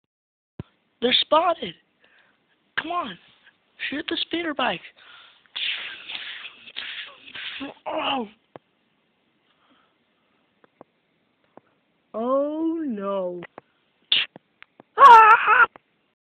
An Average Day on Hoth
a completly random video made wth a cell phone and two new lego sets